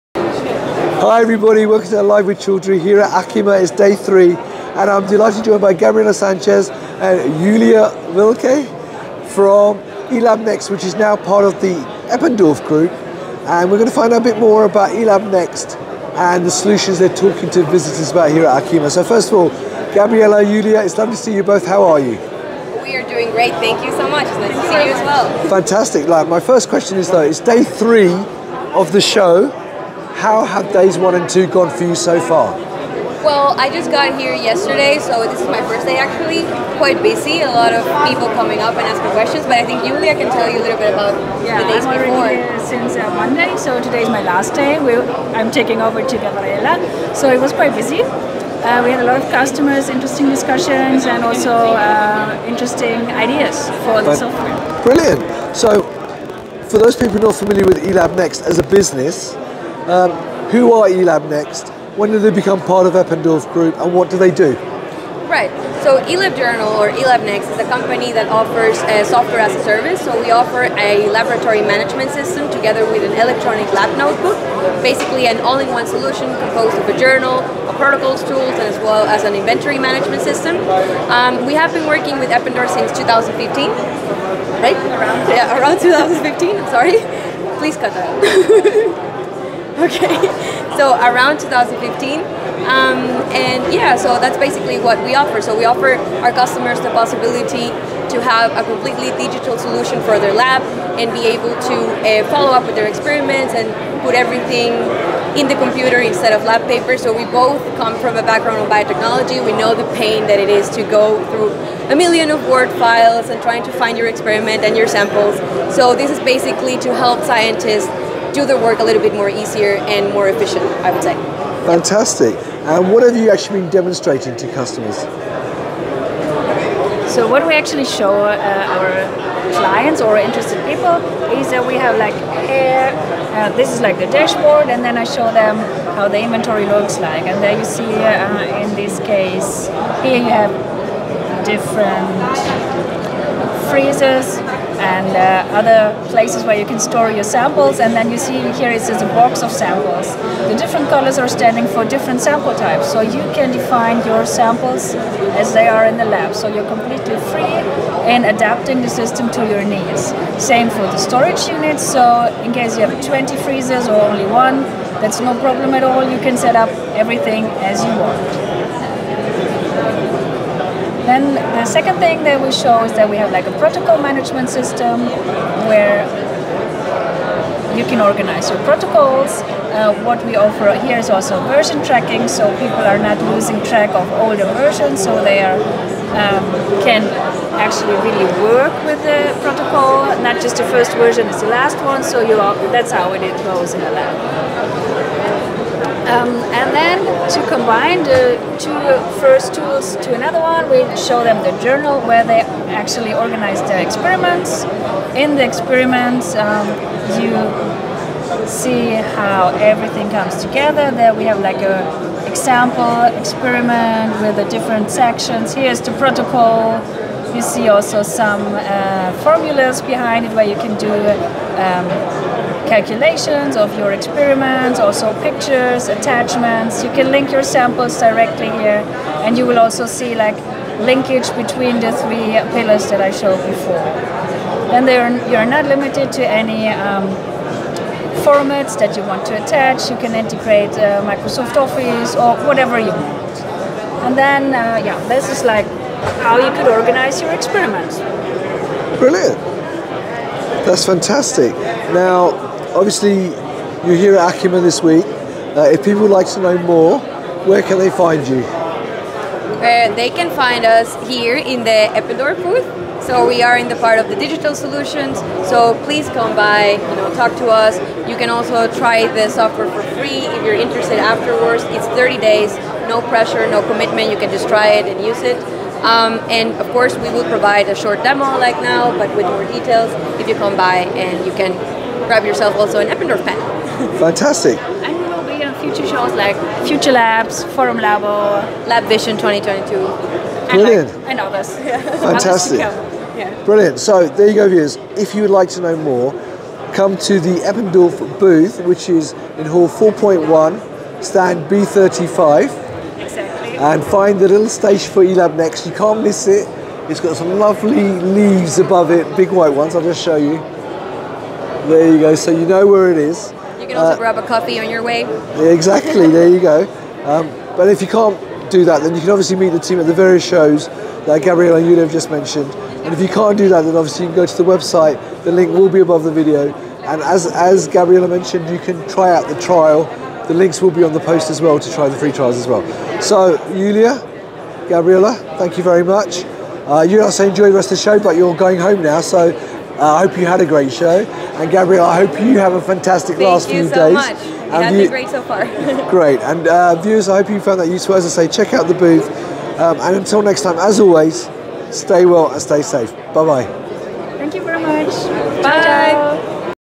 LiveWithChaudhrey with eLabNext, Eppendorf Group (Hall 4.1 booth B35) at ACHEMA 2022, Aug 24 Day 4
It was lovely to meet Gabriela and Julia from eLabNext, at the Eppendorf booth Hall 4.1 Stand B35 yesterday to learn more and get a demo of their digitallab solutions for any laboratory need, including LaboratoryManagement software and ResearchRecordsRegulation tools.

Learn more by visiting the booth in Hall 4.1 Booth B35 or ...

Andreas Konert Björn Mathes